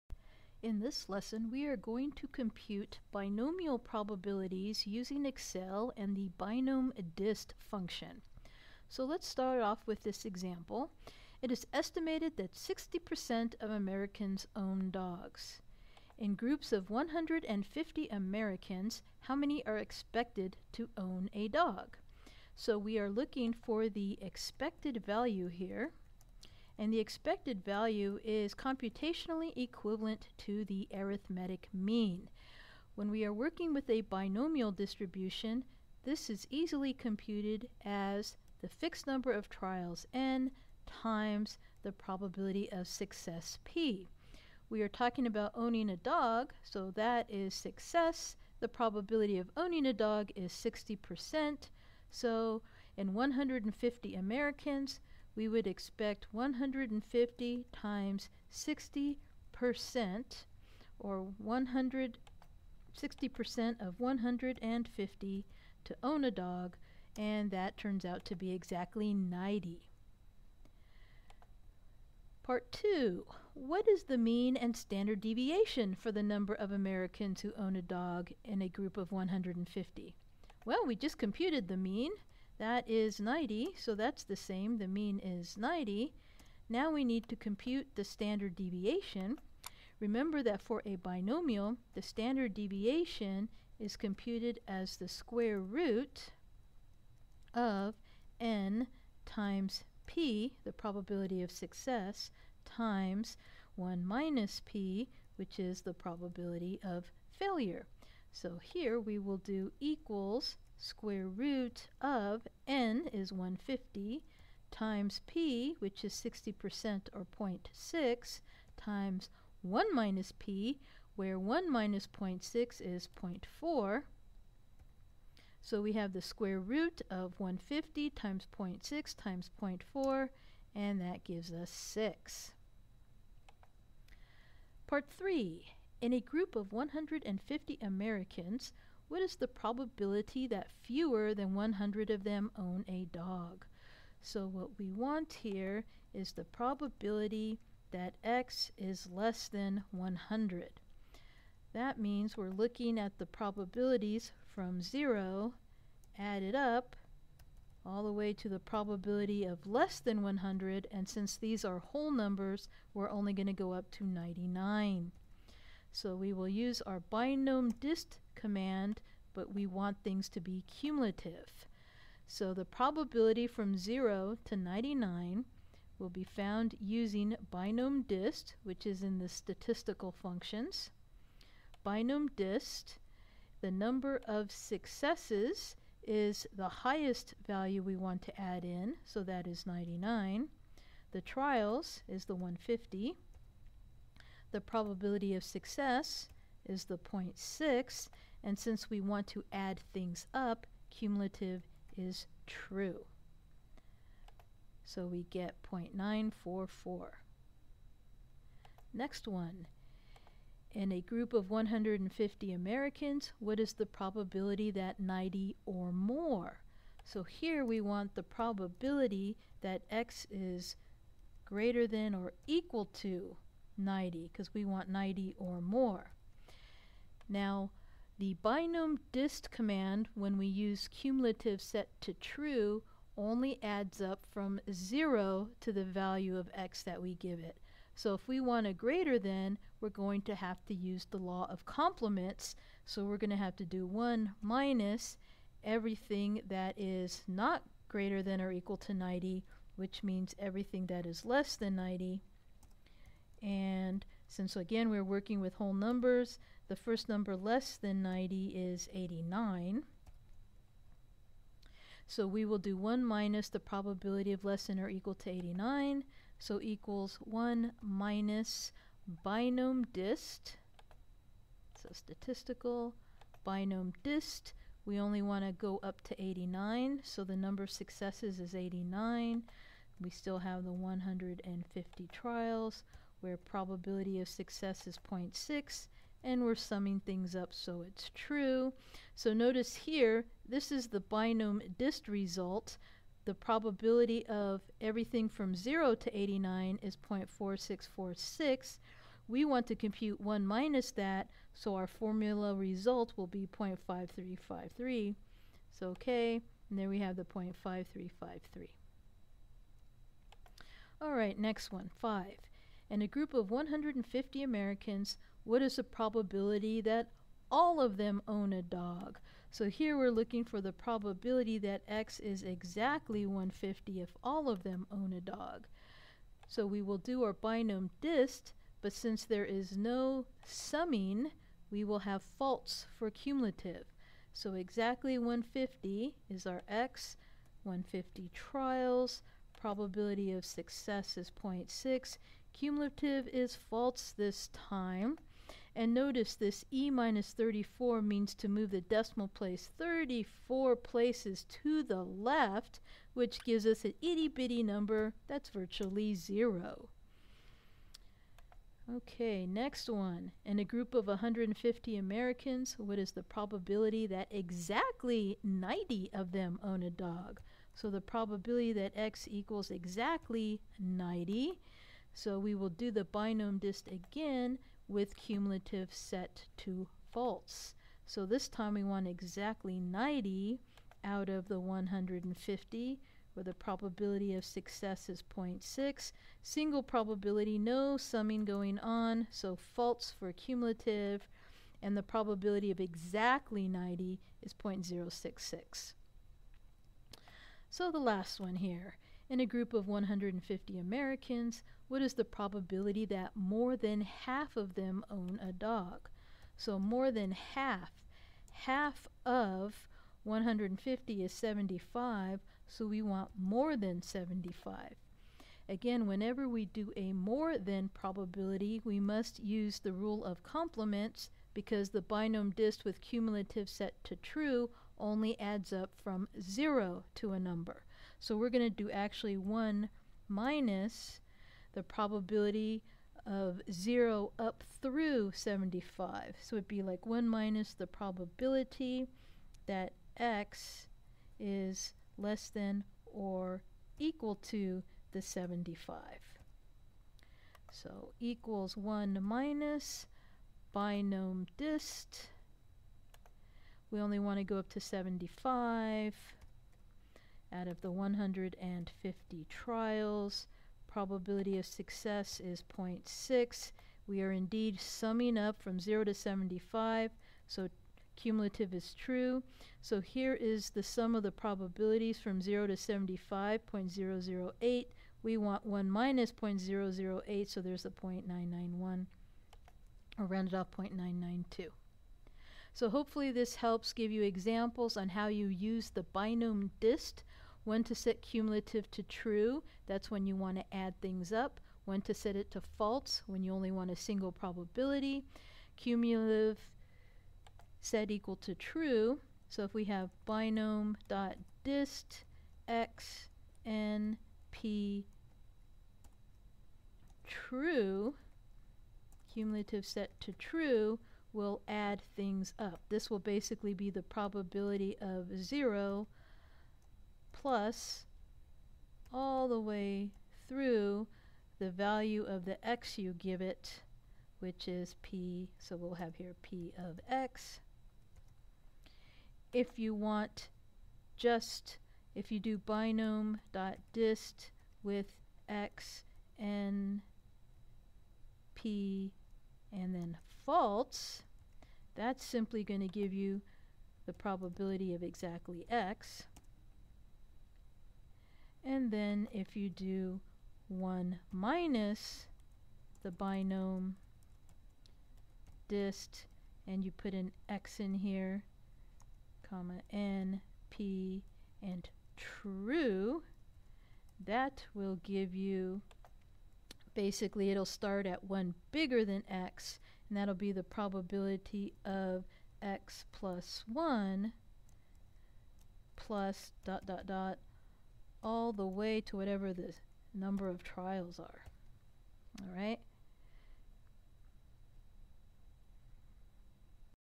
BINOMDIST with EXCEL
Computing binomial probabilities using the BINOM.DIST function in EXCEL.